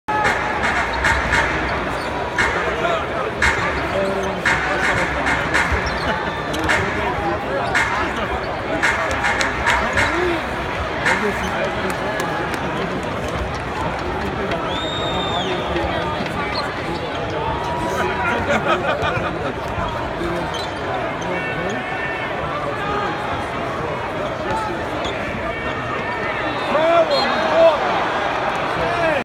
Ukraine v. Serbia - Boban Marjanovic beats Alex Len to the rebound
San Antonio Spurs vs. Phoenix Suns